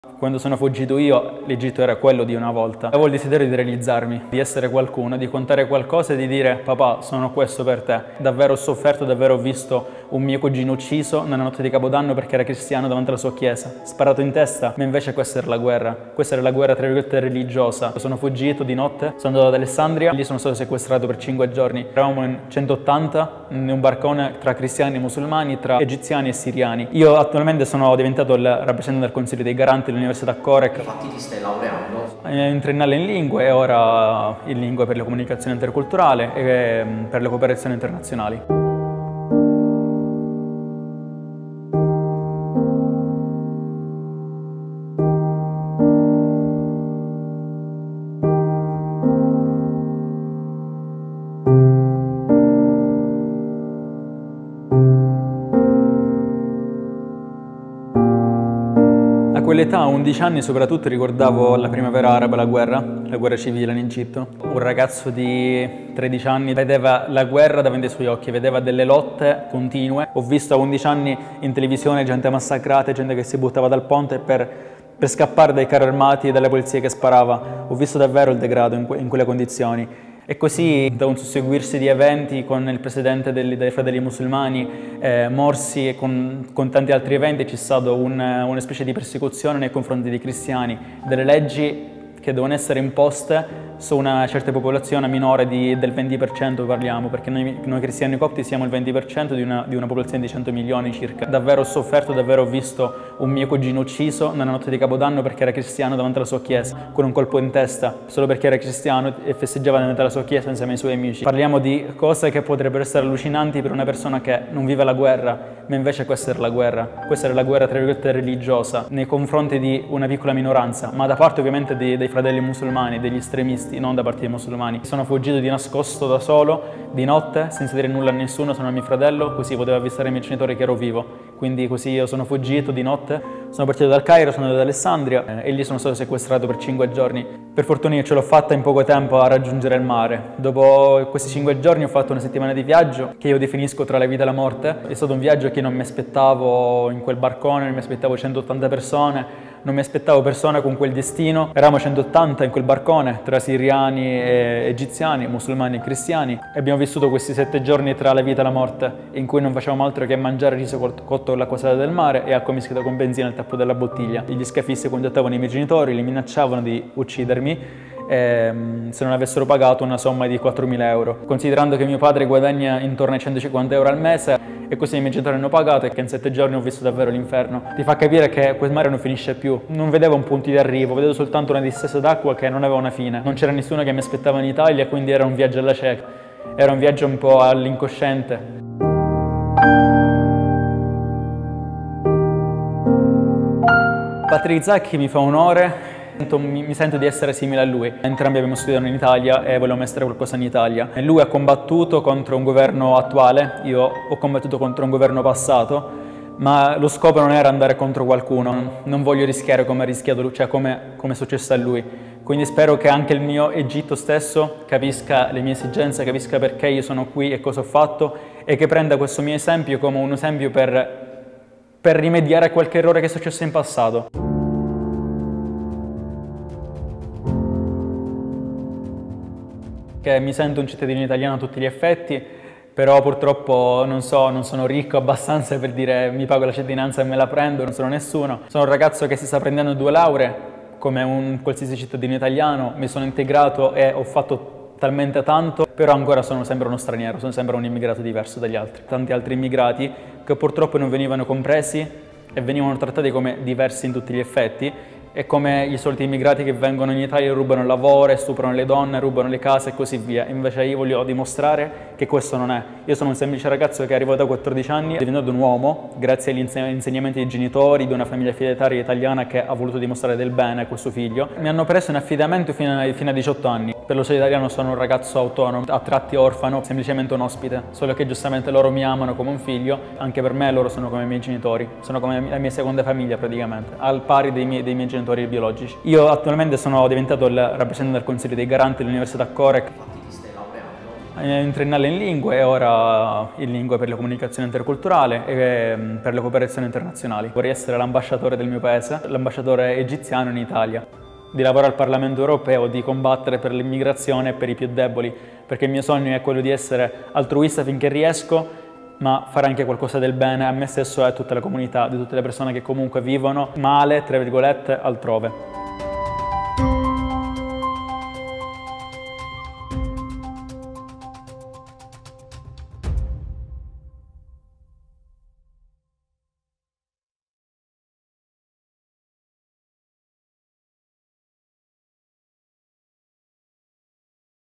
Remon, fuggito dall’Egitto a 14 anni da solo: “Ora sogno la cittadinanza italiana”
La storia di Remon Karam, giovane egiziano oggi 22enne. Una storia di resilienza, di coraggio ma anche di riscatto e voglia di vivere. Remon aveva 14 anni quando arriva in Italia con un barcone, da solo e senza la sua famiglia. Il giovane è cristiano copto e correva grossi pericoli nella sua terra reduce dalla primavera araba e dalla persecuzione cristiana. Un viaggio della speranza su un barcone tra la vita e la morte. Oggi Remon è rappresentante degli studenti dell'università siciliana e sogna di diventare Ambasciatore del suo paese.